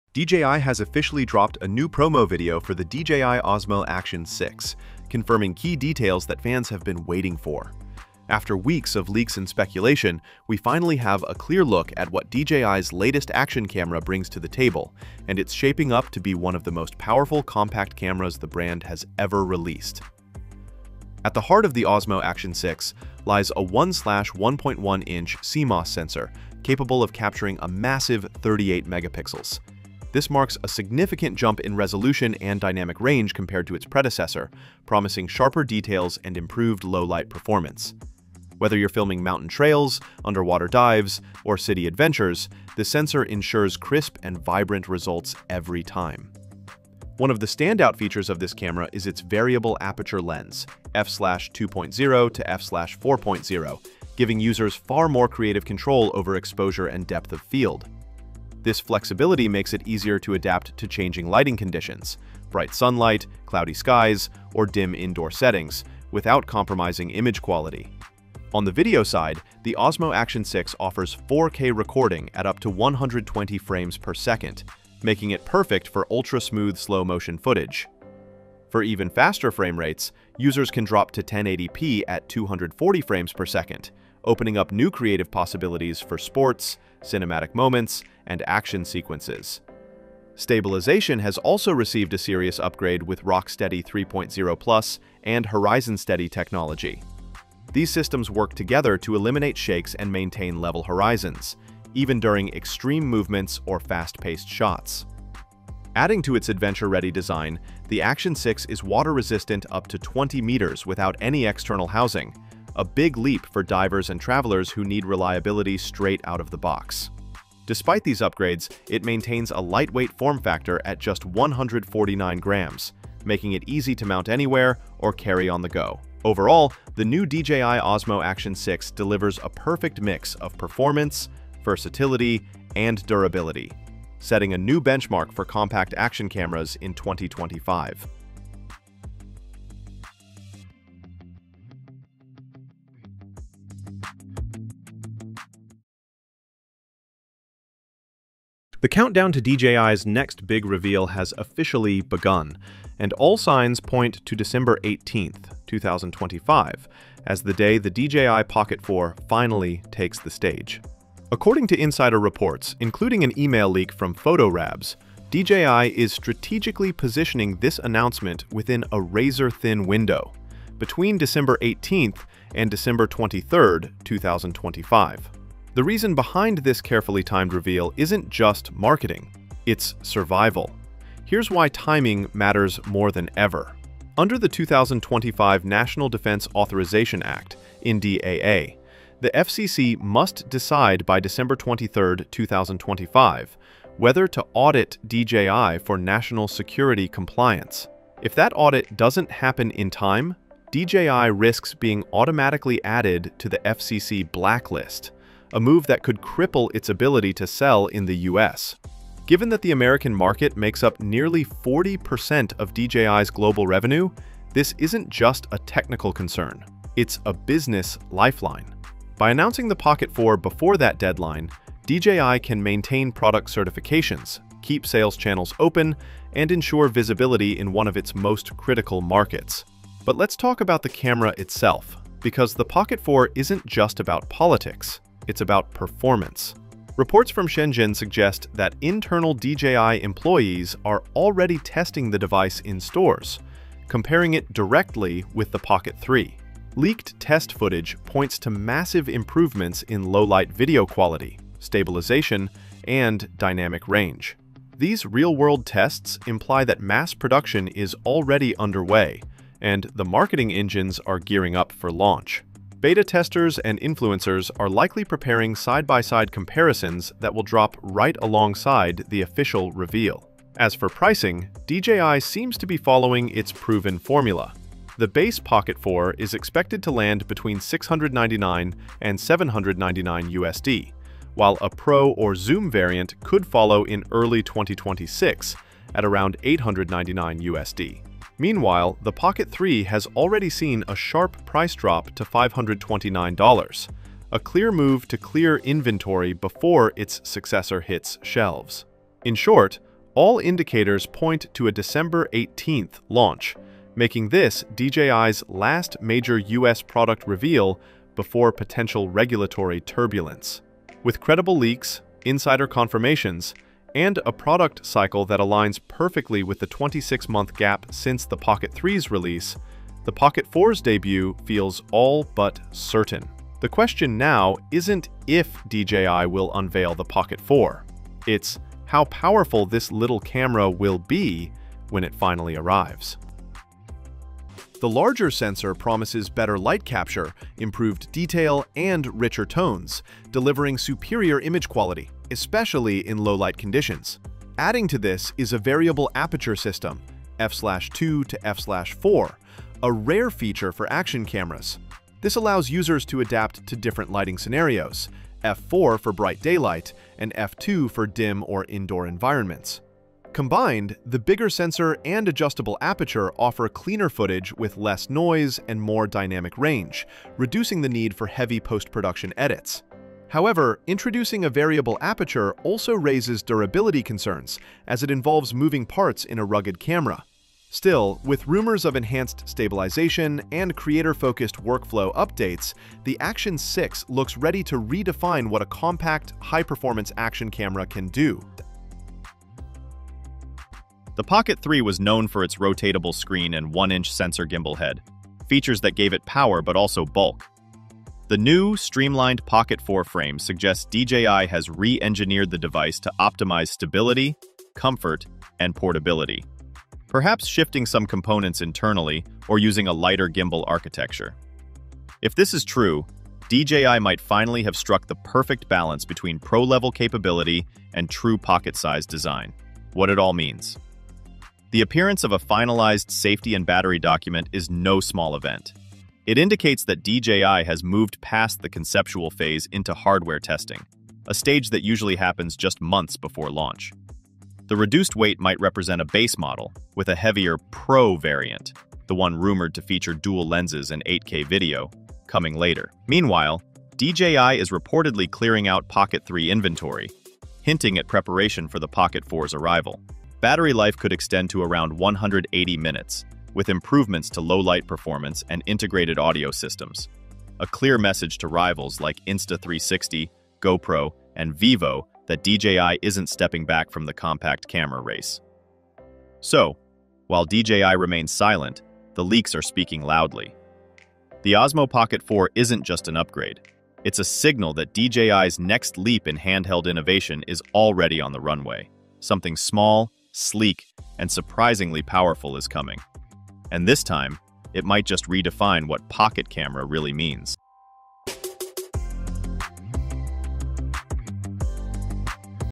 DJI Osmo Action 6 – Official Introduction & Full Specs Breakdown! 🔥
The DJI Osmo Action 6 is shaping up to be the most significant upgrade in the series so far. Early leaks point to a powerful new 1-inch sensor, 8K video recording, enhanced RockSteady 4.0 stabilization, and built-in storage. DJI appears ready to raise the bar for action cameras once again. Reports also suggest a slimmer design, improved battery life, faster performance, and early pricing details—fueling excitement ahead of its expected late-September 2025 launch.